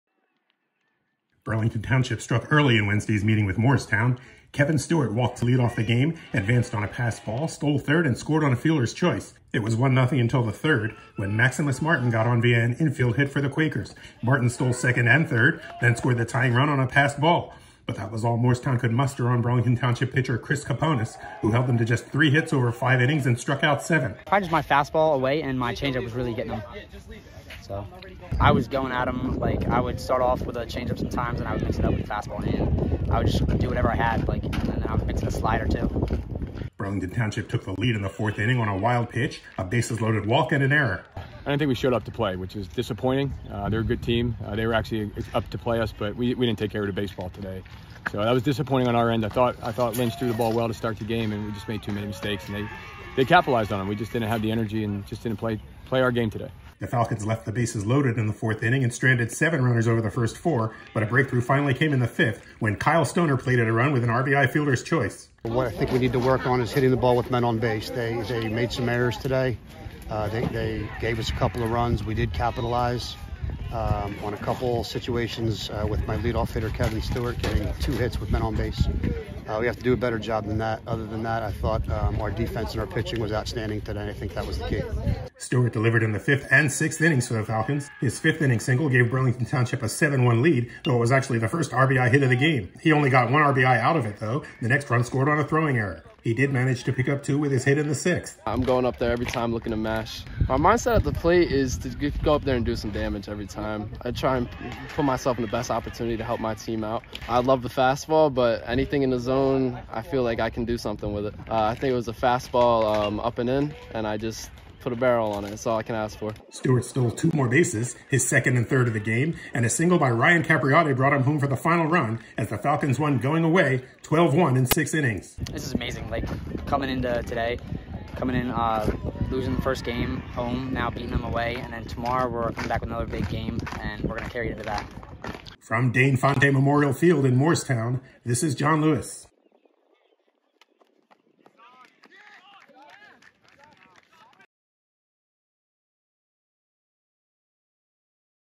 Burlington Township hammers Moorestown
Kevin Stewart had a pair of RBI hits to back the pitching of Chris Kaponis, who allowed three hits over five innings, as Burlington Township defeated Moorestown, 12-1.